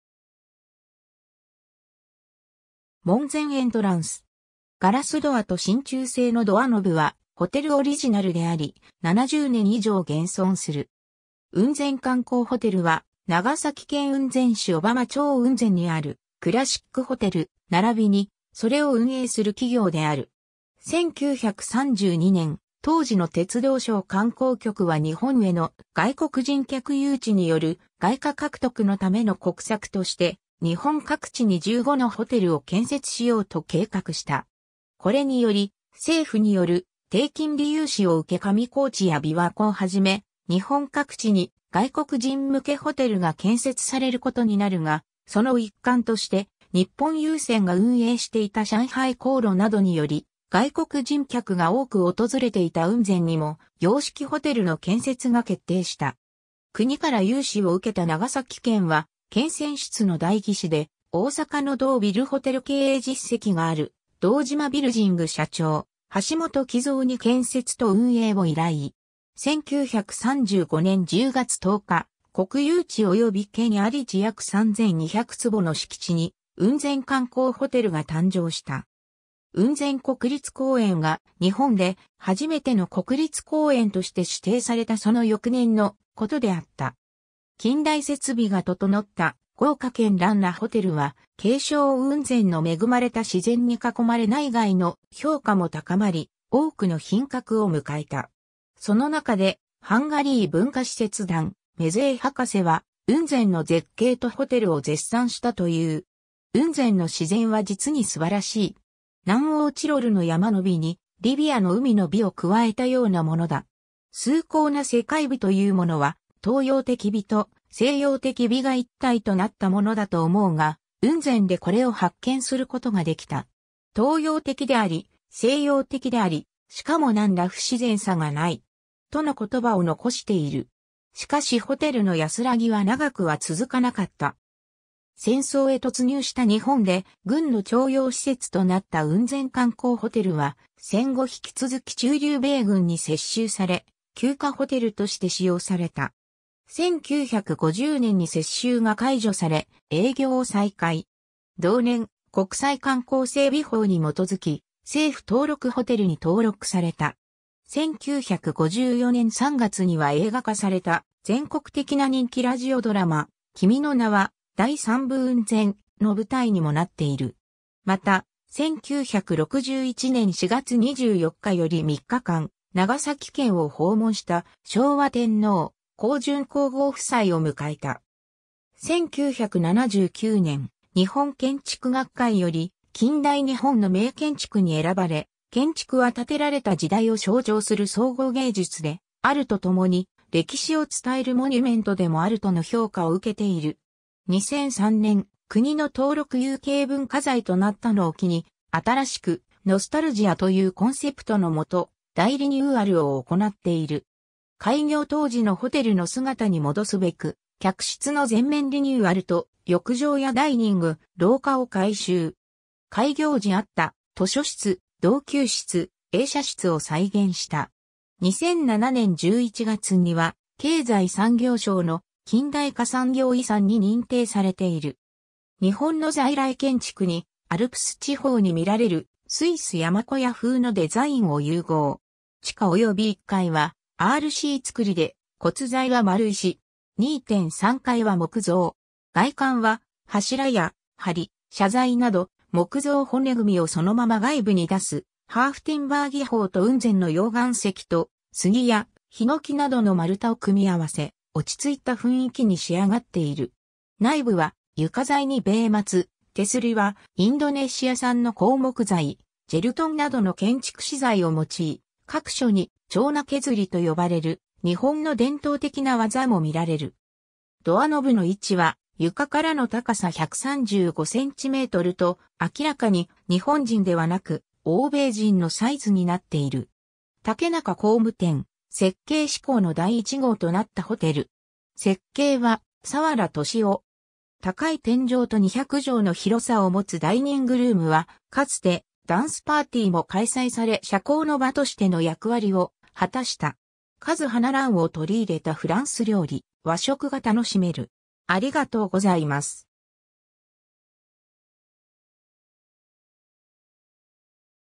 雲仙観光ホテル
門前 エントランス。
硝子ドアと真鍮製のドアノブはホテルオリジナルであり、70年以上現存する。
 雲仙観光ホテル（うんぜんかんこうホテル、英: Unzen Kanko Hotel）は、長崎県雲仙市小浜町雲仙にあるクラシックホテル、ならびにそれを運営する企業（株式会社雲仙観光ホテル）である。
 1932年（昭和7年）、当時の鉄道省観光局は日本への外国人客誘致による外貨獲得のための国策として、日本各地に15のホテルを建設しようと計画した。
これにより政府による低金利融資を受け上高地や琵琶湖をはじめ、日本各地に外国人向けホテルが建設されることになるが、その一環として、日本郵船が運営していた上海航路などにより外国人客が多く訪れていた雲仙にも洋式ホテルの建設が決定した。
 国から融資を受けた長崎県は、県選出の代議士で大阪の堂ビルホテル経営実績がある堂島ビルヂング社長・橋本喜造に建設と運営を依頼。
1935年（昭和10年）10月10日、国有地及び県有地約3,200坪の敷地に雲仙観光ホテルが誕生した。
雲仙国立公園が日本で初めての国立公園として指定されたその翌年のことであった。
 近代設備が整った豪華絢爛なホテルは、景勝雲仙の恵まれた自然に囲まれ内外の評価も高まり、多くの賓客を迎えた。
その中で、ハンガリー文化使節団メゼイ博士は雲仙の絶景とホテルを絶賛したという。
「雲仙の自然は実に素晴しい。南欧チロルの山の美にリビアの海の美を加えたようなものだ。崇高な世界美というものは、東洋的美と西洋的美が一体となったものだと思うが、雲仙でこれを発見することができた。東洋的であり、西洋的であり、しかも何ら不自然さがない。（1938年2月27日・大阪朝日新聞所蔵）」との言葉を残している。
 しかしホテルの安らぎは長くは続かなかった。
戦争へ突入した日本で、軍の徴用施設となった雲仙観光ホテルは、戦後引き続き駐留米軍に接収され休暇ホテルとして使用された。
1950年（昭和25年）に接収が解除され、営業を再開。
同年、国際観光整備法に基づき、政府登録ホテル（政府登録国際観光旅館）に登録された。
（第ホ29号） 1954年（昭和29年）3月には映画化された全国的な人気ラジオドラマ「君の名は」―第3部雲仙―」の舞台にもなっている。
また、1961年（昭和36年）4月24日より3日間、長崎県を訪問した昭和天皇・香淳皇后夫妻を迎えた。
1979年（昭和54年）、日本建築学会より近代日本の名建築に選ばれ、建築は建てられた時代を象徴する総合芸術であると共に、歴史を伝えるモニュメントでもあるとの評価を受けている。
 2003年（平成15年）、国の登録有形文化財となったのを機に、「新しくノスタルジア」というコンセプトのもと、大リニューアルを行っている。
開業当時のホテルの姿に戻すべく、客室の全面リニューアルと浴場やダイニング、廊下を改修。
開業時あった図書室・撞球室、映写室を再現した。
 2007年（平成19年）11月には、経済産業省の「近代化産業遺産」に認定されている。
 日本の在来建築にアルプス地方に見られるスイス山小屋風のデザイン（ハーフティンバー様式）を融合。
地下及び１階はRC造で、骨材は丸石。
2・3階は木造。
外観は、柱や梁（はり）、斜材など木造骨組をそのまま外部に出すハーフティンバー技法と雲仙の溶岩石と杉やヒノキなどの丸太を組み合わせ、落ちついた雰囲気に仕上がっている。
 内部は、床材に米松、手摺りはインドネシア産の硬木材・ジェルトン（Dyera spp., 主にクワガタノキ D. costulata）などの建築資材を用い、各所に手斧（ちょうな）削りと呼ばれる日本の伝統的な技も見られる。
ドアノブの位置は床からの高さ135cmと、明らかに日本人ではなく欧米人のサイズになっている。
竹中工務店、設計施工の第一号となったホテル。
設計は、早良俊夫。
 「高い天井と200畳の広さを持つダイニングルームは、かつてダンスパーティーも開催され社交の場としての役割を果たした。」 和華蘭を取り入れたフランス料理、和食が楽しめる。